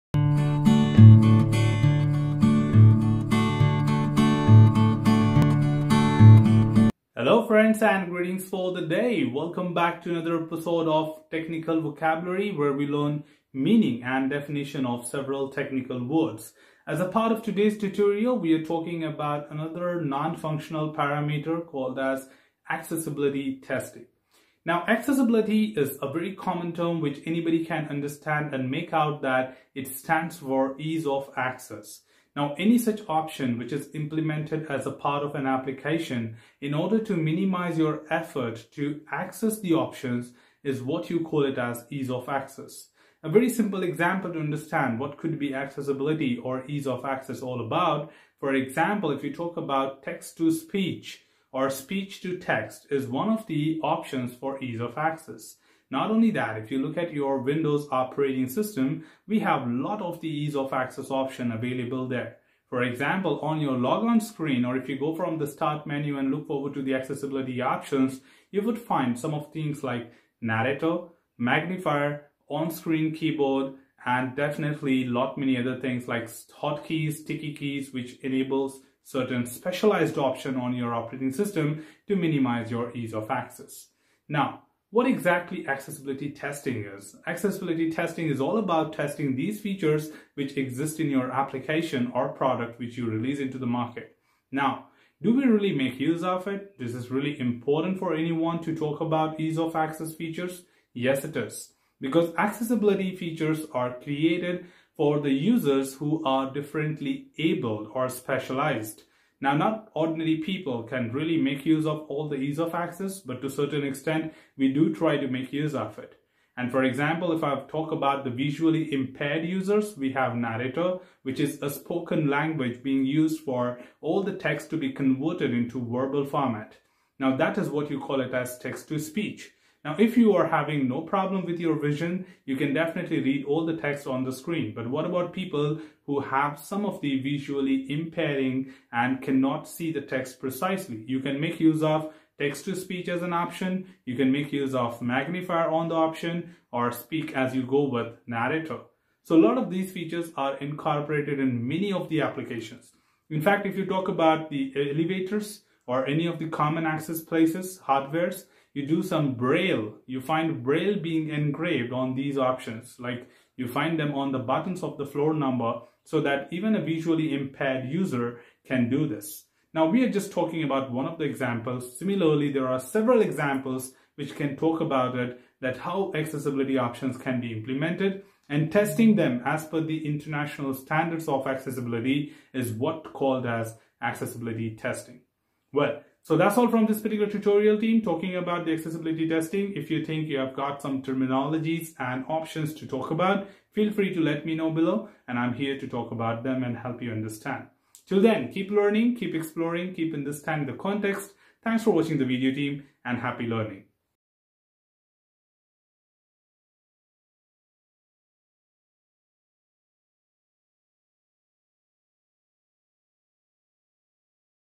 Accessibility Testing | Meaning of Accessibility Testing | Definition of Accessibility Testing
Hello Friends,

This channel is all about providing basic definition, meaning and example of various terms used in technical world.

This video will help you in understanding what is Accessibility Testing.